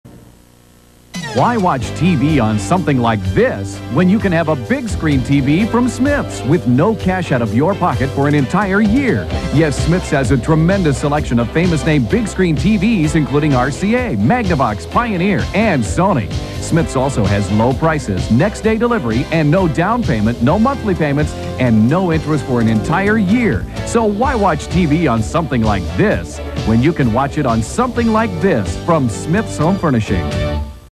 1991 Smith's Home Funishings "A big screen with no cash out of pocket" Portland Local TV Commercial
This commercial aired on January 13th, 1991. It appeared during a broadcast "Good Grief" on KPDX Channel 49.